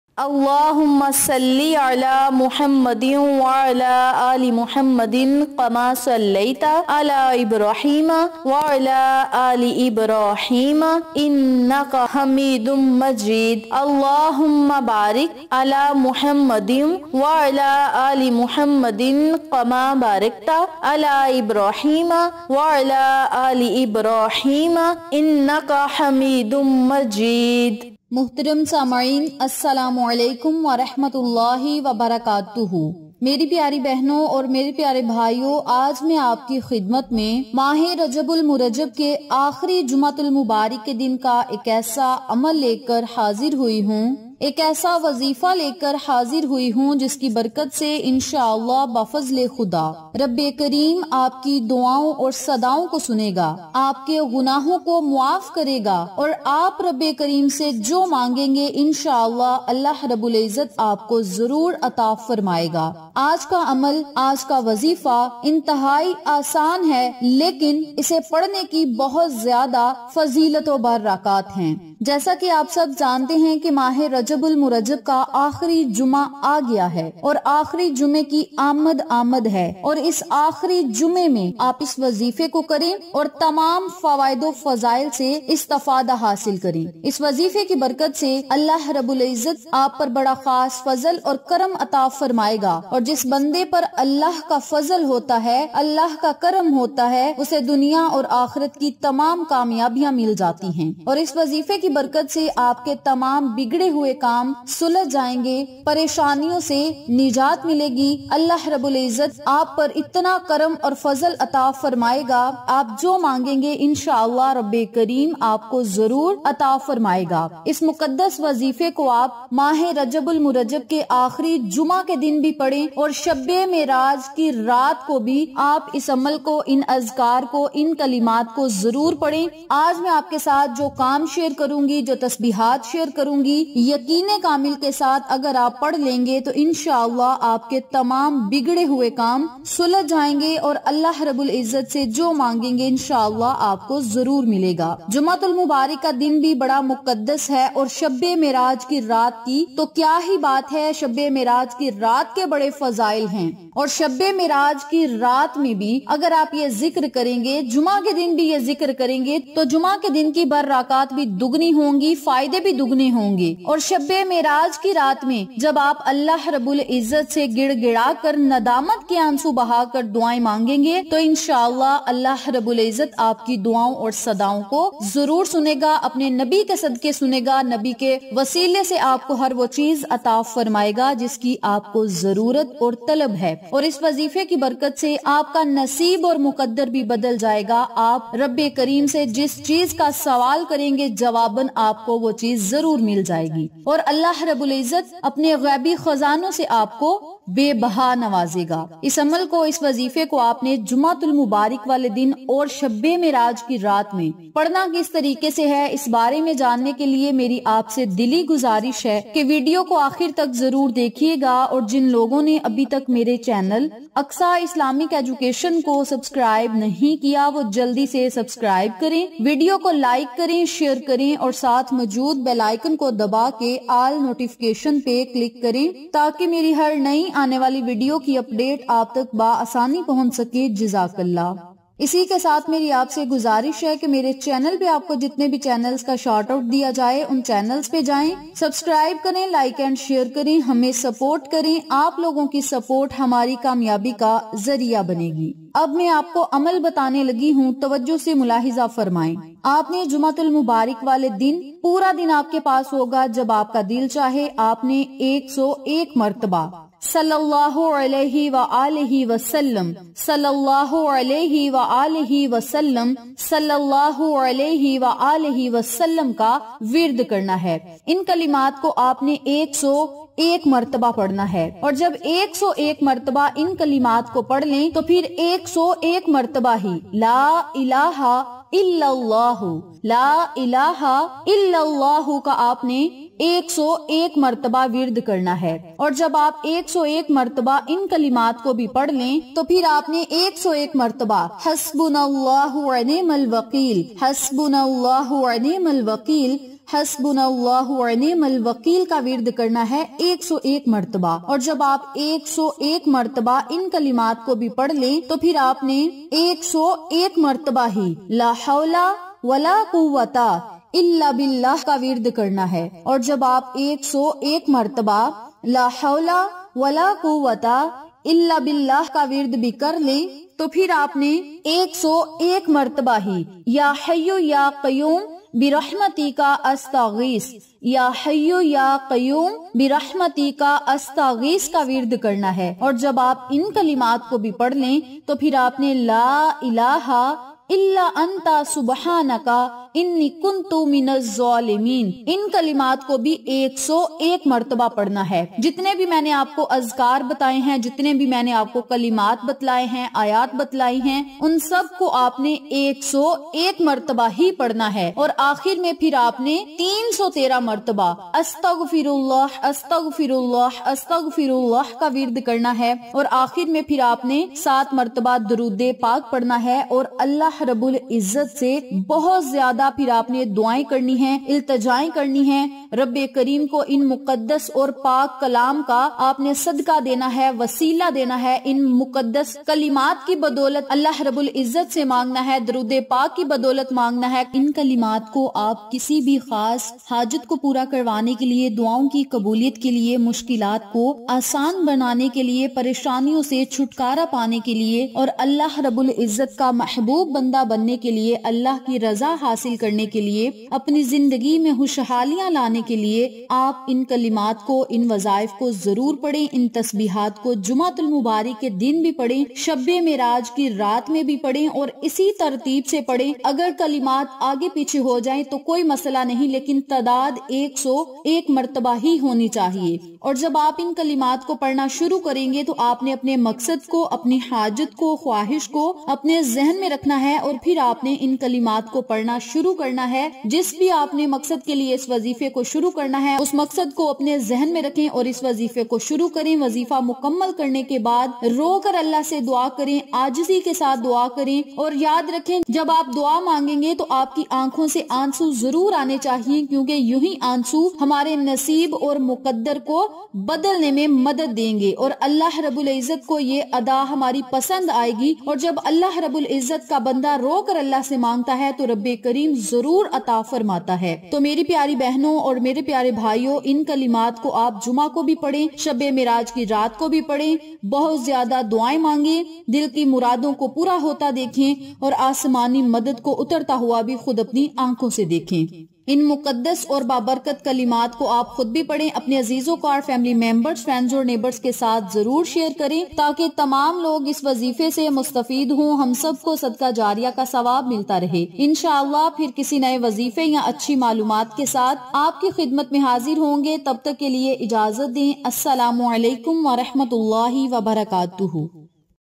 5 Beautiful Things On Last friday of rajab and Shab e Meraj 2023 | 27 Rajab ki Raat 5 kam zoror karn
5 Beautiful Things On Last friday of rajab and Shab e Meraj 2023 | 27 Rajab ki Raat 5 kam zoror karn

Welcome to this video, Today we are going to share with you the virtue of last friday and Shab Miraj and the Wazifa of Shab Miraj. Shab Miraj is a very auspicious night. You must recite three prayers on the night of Ascension. Allah will fulfill every need. Success will be found in every goal. This is the blessed night of Shab Miraj 2023. Whatever you ask Allah in this night, you will surely get it. in Sha Allah